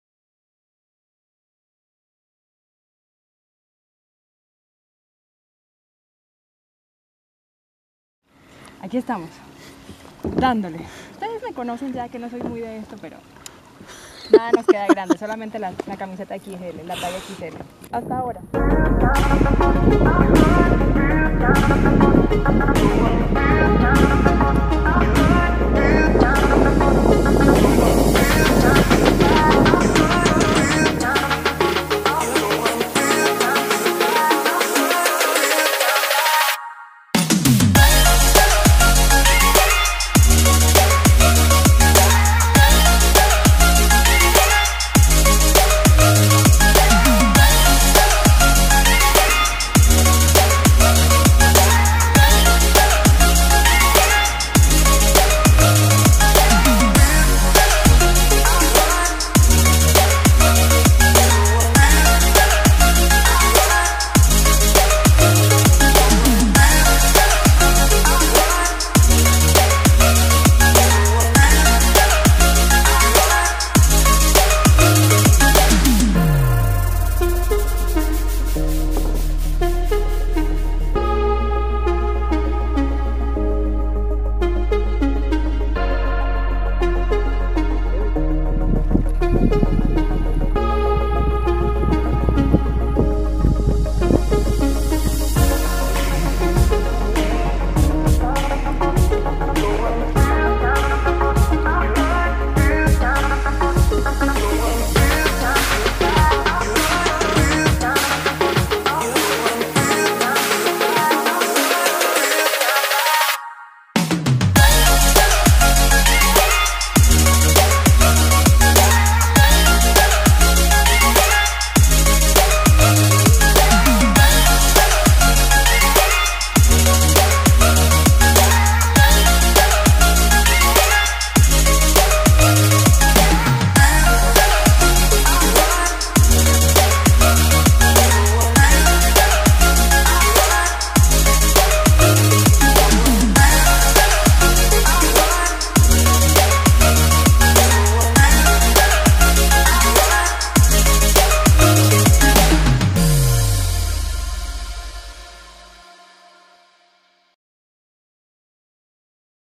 Nueva aventura desbloqueada (kayaking Guatapé)
Remar en kayak de travesía es una forma de remar muy placentera; aprender la técnica, moverse en el agua, conocer nuestro cuerpo y disfrutar la vida desde otro punto de vista, es una experiencia que debes permitirte vivir. Ven a remar en kayaks de travesía a Guatapé.